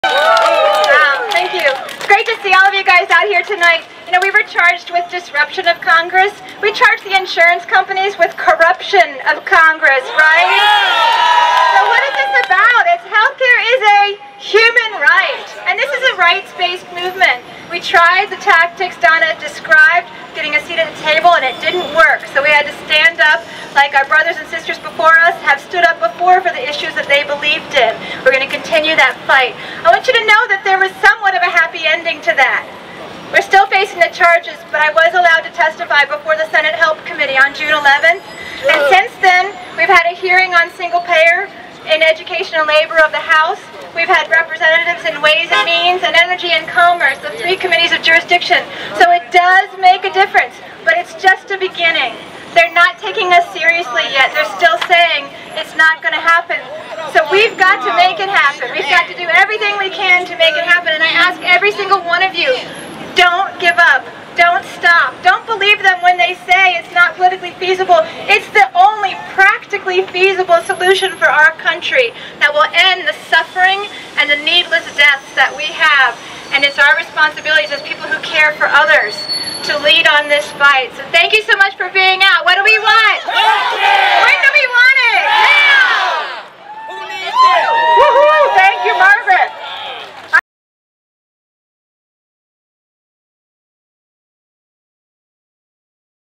Rally for Single Payer at Union Station, in Washington, D.C.
Activists for a single payer health care system staged a lively rally in front of historic Union Station, in Washington, D.C., on Friday evening, June 26, 2009. Dr. Margaret Flowers of Baltimore, MD was one of the speakers. She said: "Health care is a Human Right. And, this is a Rights based movement...Dont give up, the fight," on single payer... "We've got to make it happen. Dont stop!"
The healthcare activists are supporting HR 676 and S 703, now pending before the U.S. Congress. Dr. Flowers was one of the "Baucus Eight," who were arrested on May 5, 2009, in the Dirksen Office Building, on Capitol Hill, for speaking out at a Senate Finance Committee. Its chairman, Sen. Max Baucus (D-MT), had arbitrarily barred single payer champions from participating in the healthcare debate.